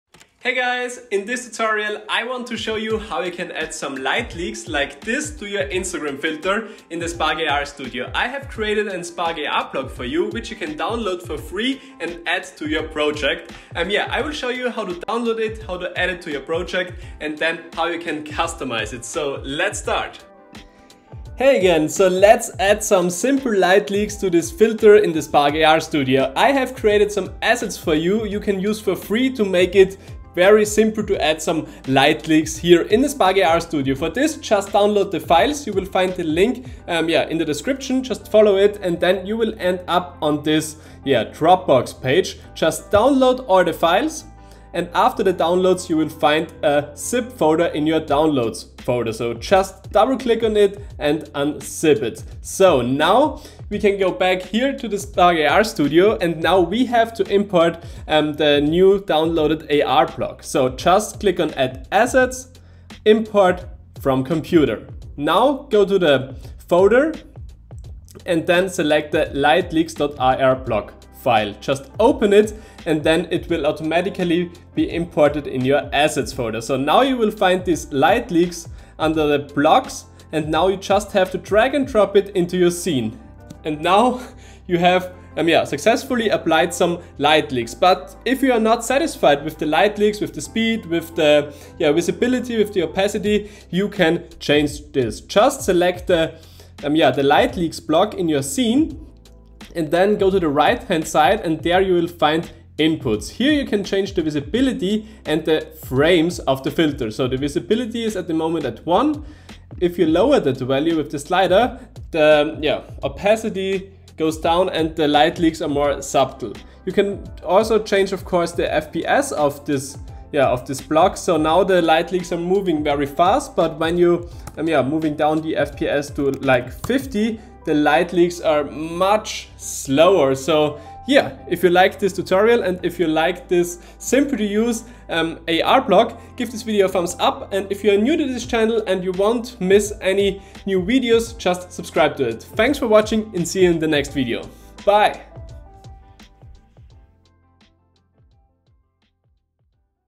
Light Leak Effect + FREE ASSETS! ✨ | Spark AR Studio Tutorial for Instagram Filters
In this Spark AR Studio Tutorial I will show you, how you can add a light leak effect to your Instagram Filters. + Free Assets!